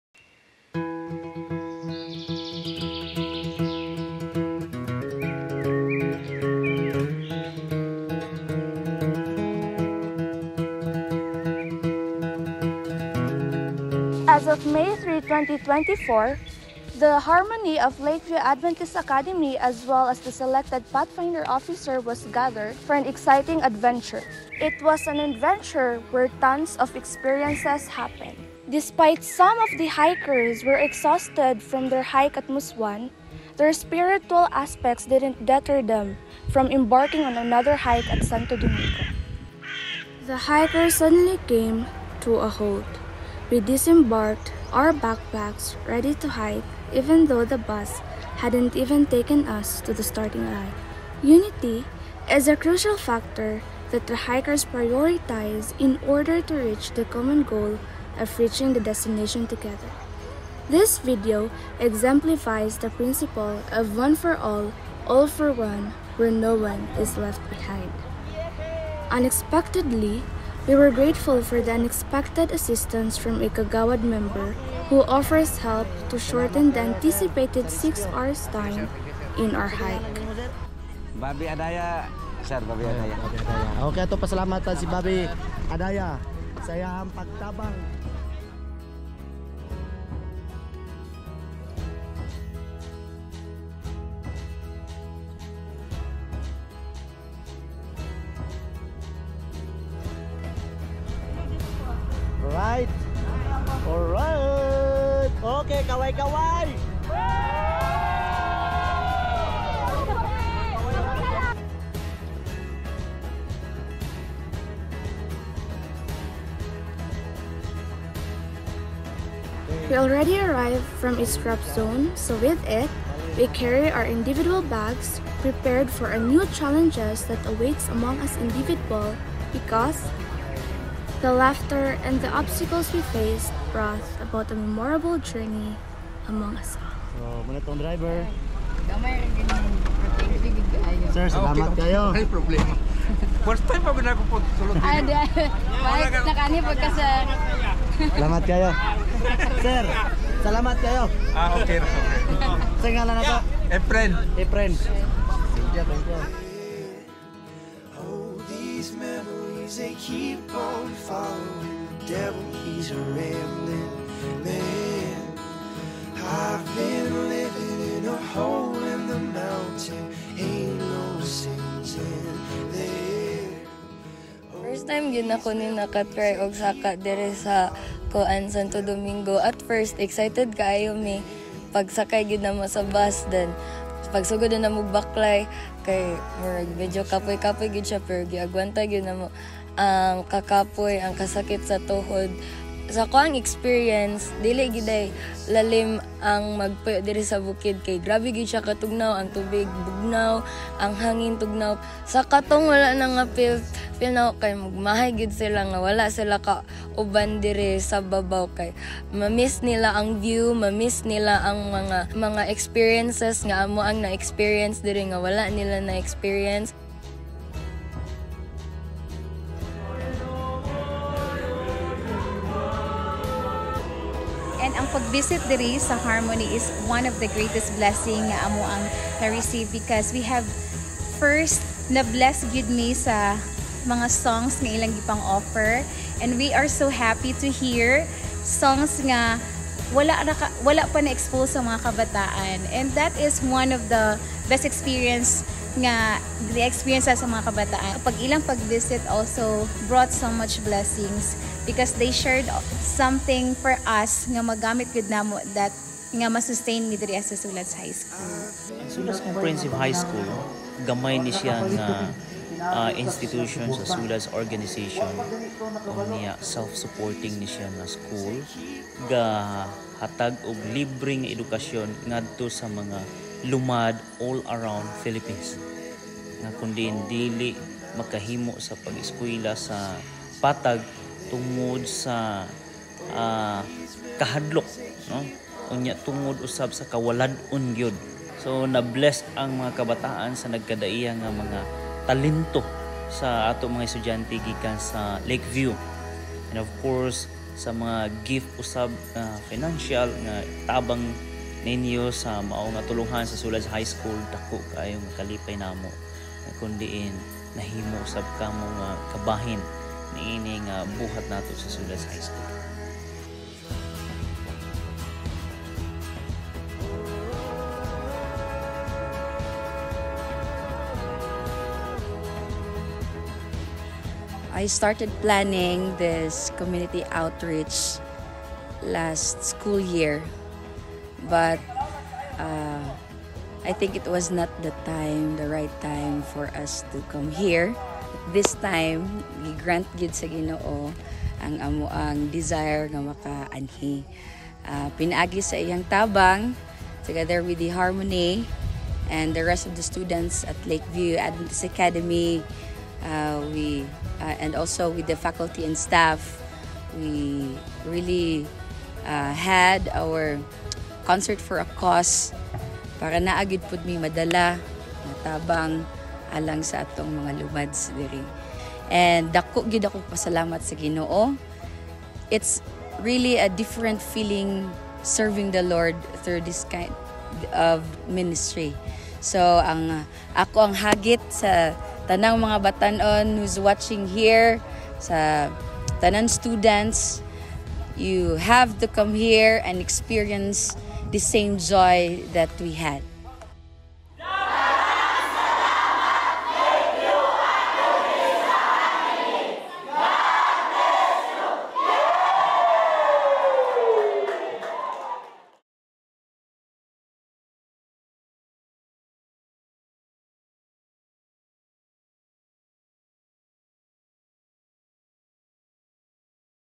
HIKING SULADS HIGH SCHOOL
Hiking and Community Outreach at SULADS Comprehensive High School, Sto. Domingo, Quezon, Bukidnon.
The LVAA Harmony together with the Pathfinder officers and several faculty members, engaged in hiking and community outreach at SULADS Comprehensive High School in Sto. Domingo, Quezon, Bukidnon, where they stayed at the school for 2 nights and 3 days. During their stay, they were able to celebrate a splendid Sabbath together with the students from SULADS Comprehensive High School.
The primary aim of this activity was to assist the students and provide donations and goods. Their visit wasn't solely for recreational purposes; they also aimed to uplift the school community with their harmonic voices. Through their participation, the LVAA Harmony members were able to offer their assistance and share their God-given talents with others.